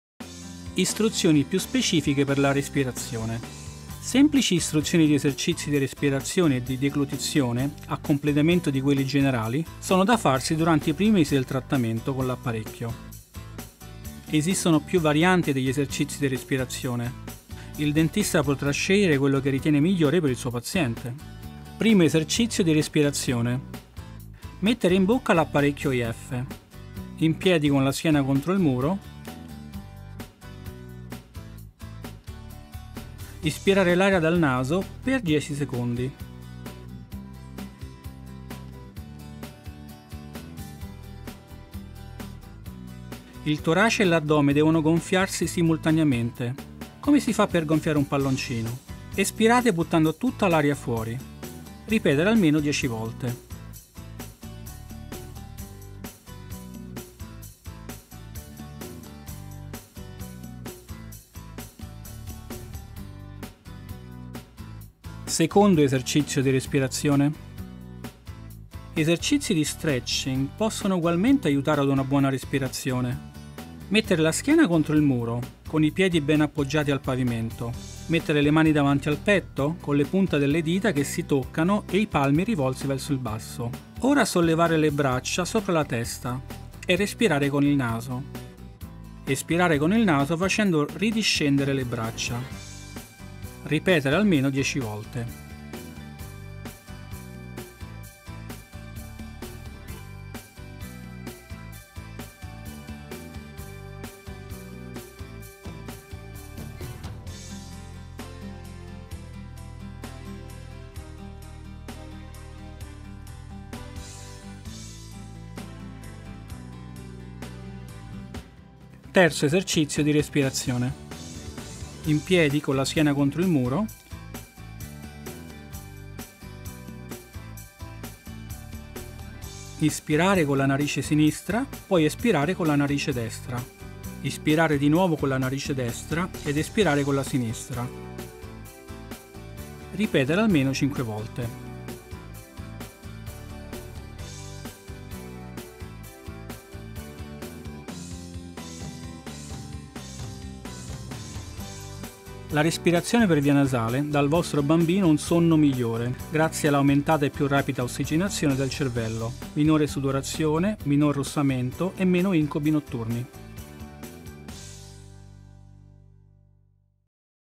3 - Educazione Funzionale: istruzioni per gli esercizi di respirazione specifici - EF Line
Video che illustra gli esercizi specifici di respirazione, da eseguire mentre si indossa l’apparecchio.